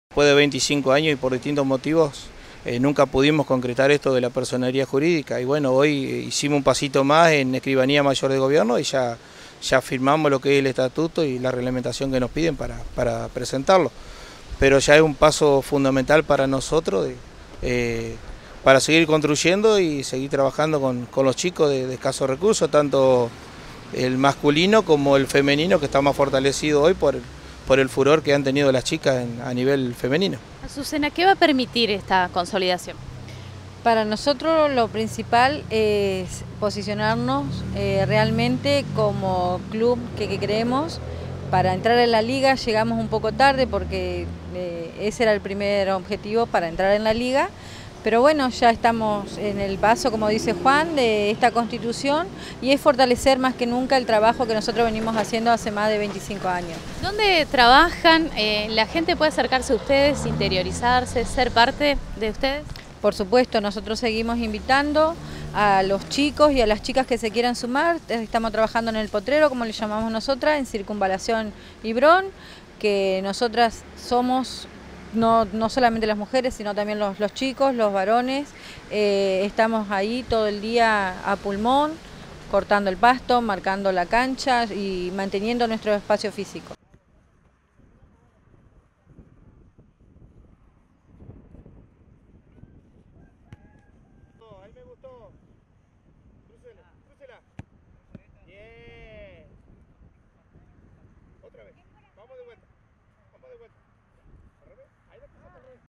ALMIRANTE BROWN
BROWN A UN PASO DE SER CLUB
AVANZAN EN LOS TRÁMITES PARA LA PERSONERÍA JURÍDICA